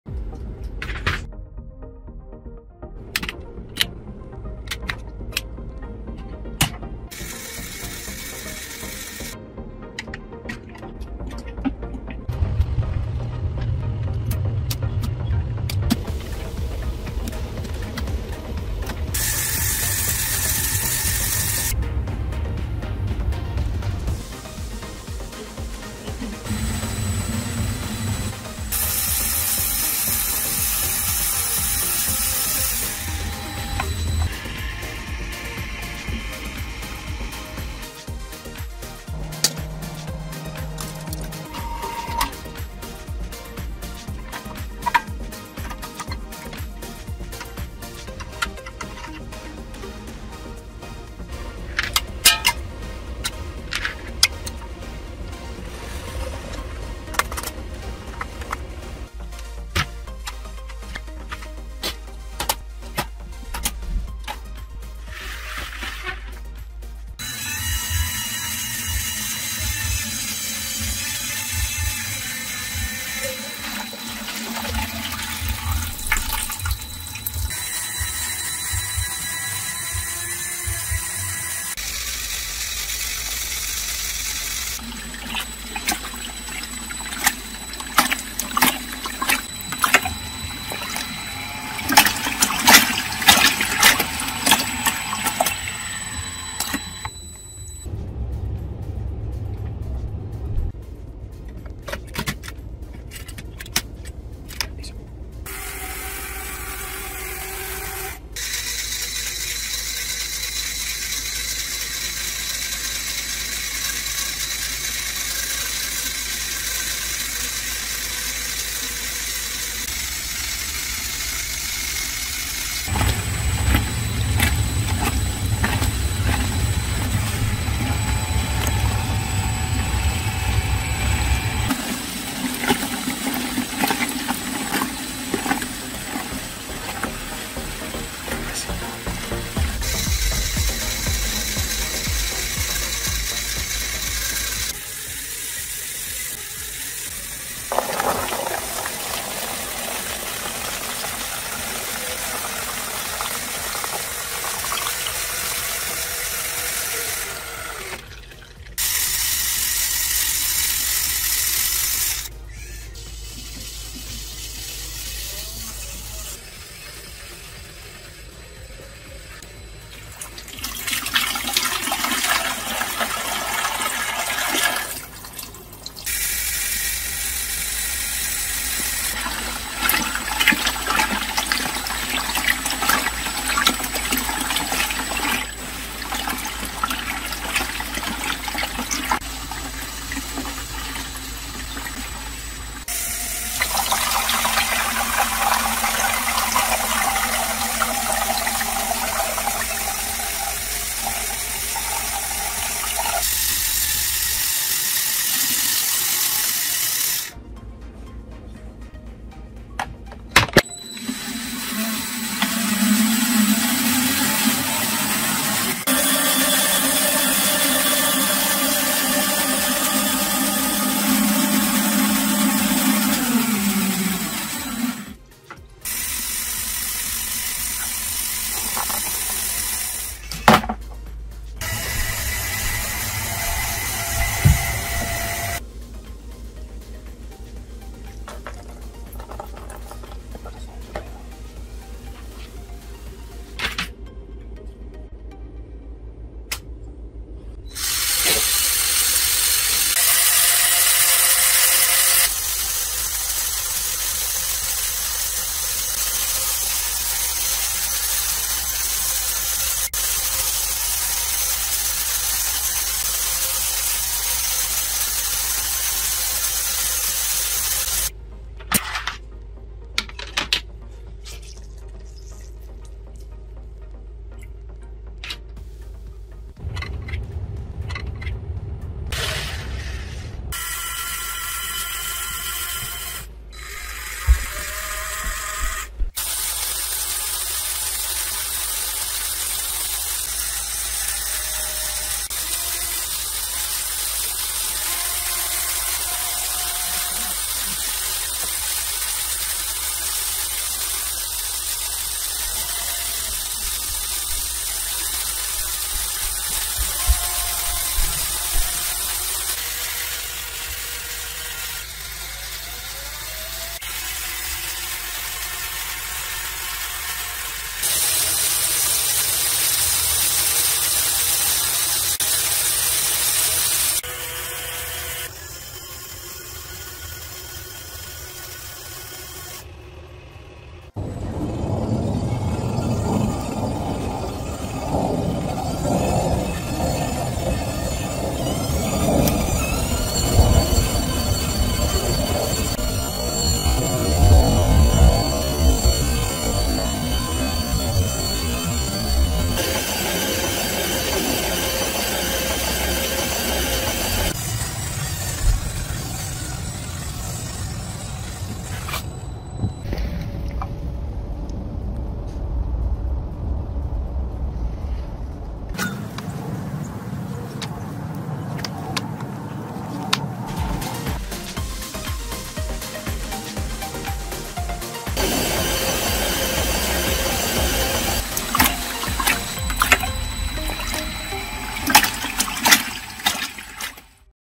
Lego Washing Machine
Student dream - working personal Lego Washing Machine.